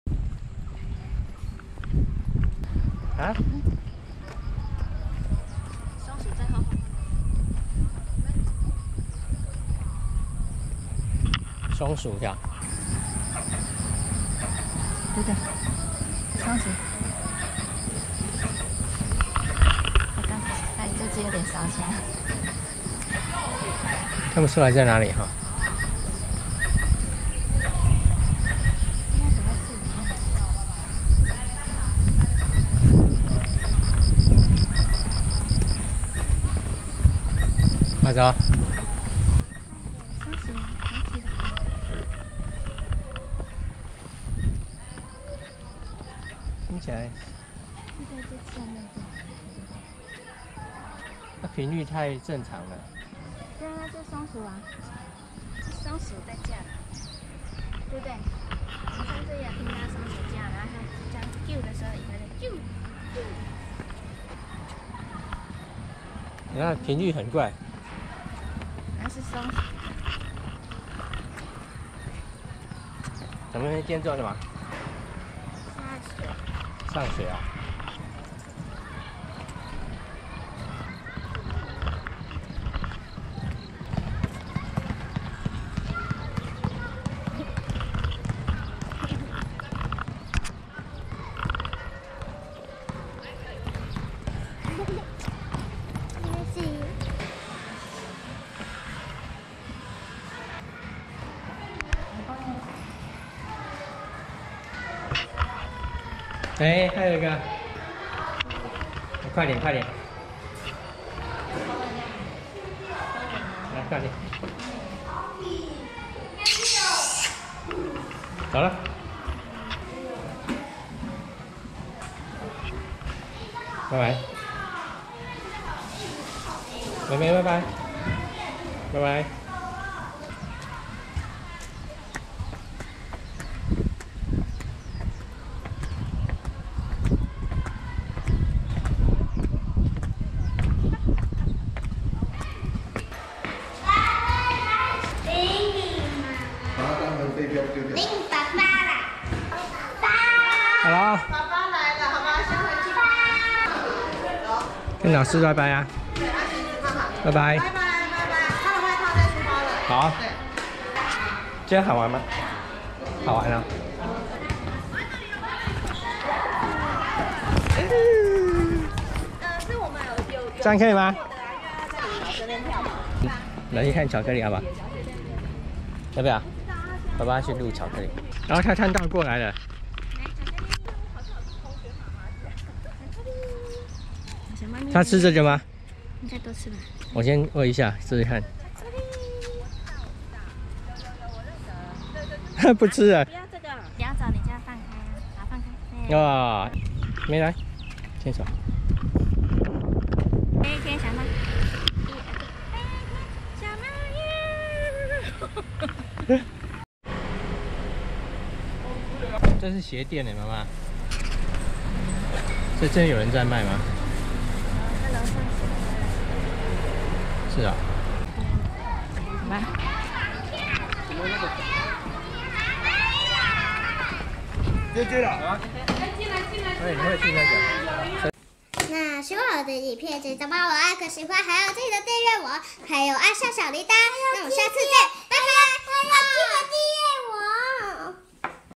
[生活日常]八斗國小送妹妹上學聽到松鼠的叫聲_190524
今天帶妹妹上學,夫人說這是松鼠的叫聲